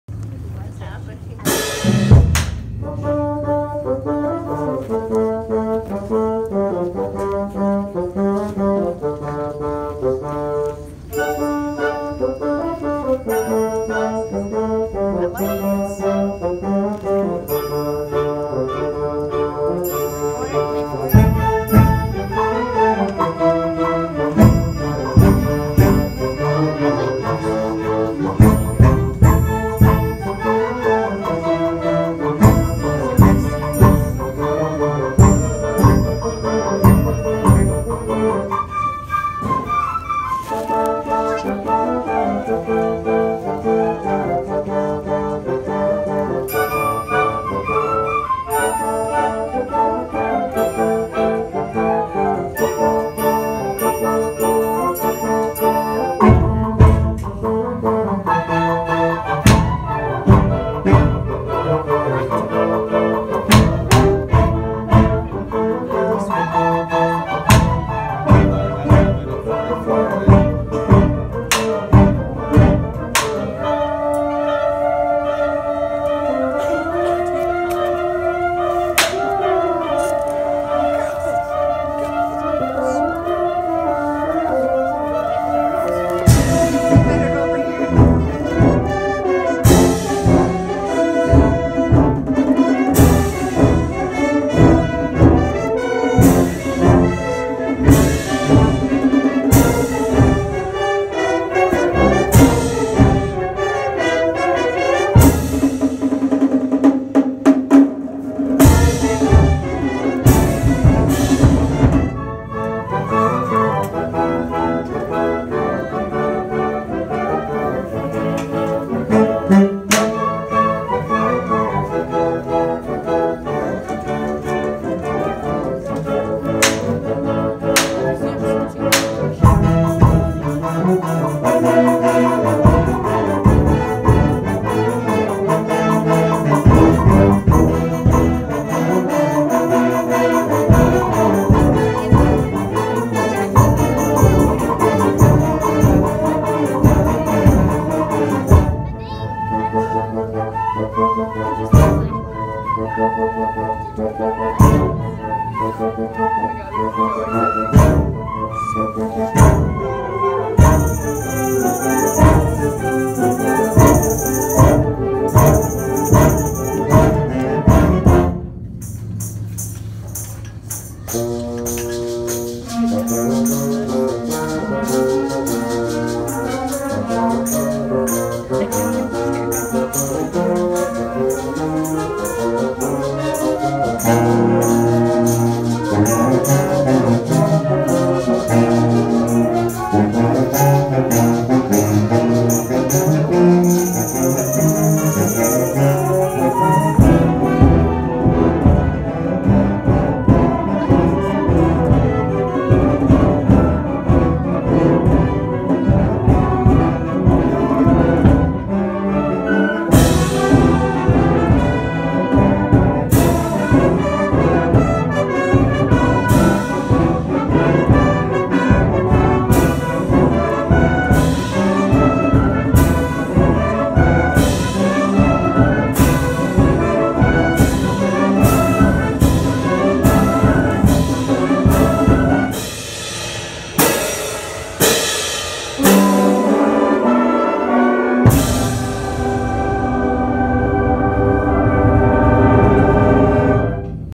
High school band
I’m not in high school but they came today and it was awesome 👏🏼👏🏼👏🏼